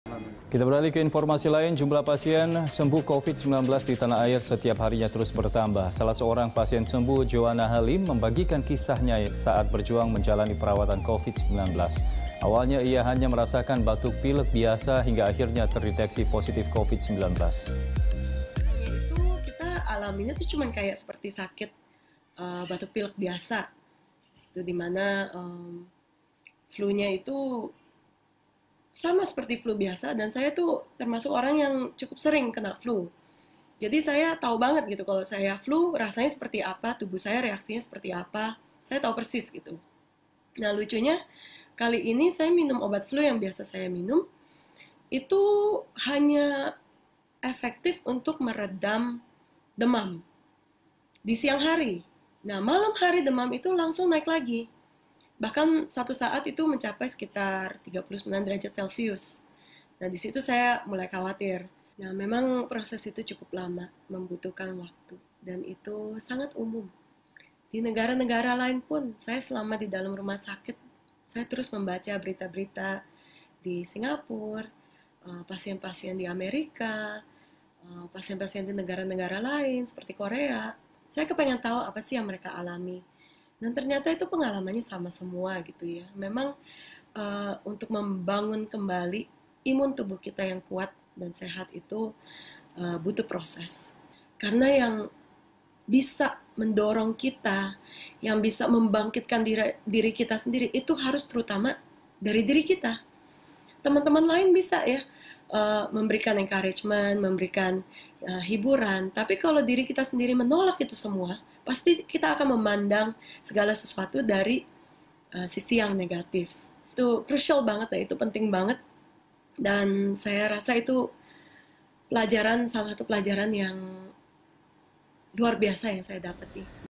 Sembuh Corona, Joanna Halim Ceritakan Pengalamannya
Jumlah pasien sembuh covid-19 di Tanah Air setiap harinya terus bertambah. Salah seorang pasien sembuh Joanna Halim membagikan kisanya saat berjuang menjalani perawatan covid-19. Awalnya, ia hanya merasakan batuk pilek biasa hingga akhirnya terdeteksi positif covid-19.